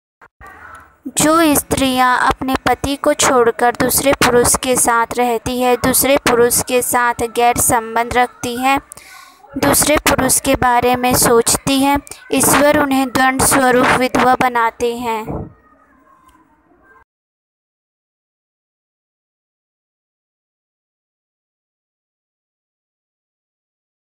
स्त्रियों के विधवा होने का कारण
किस पाप के कारण स्त्रिया विधवा होती है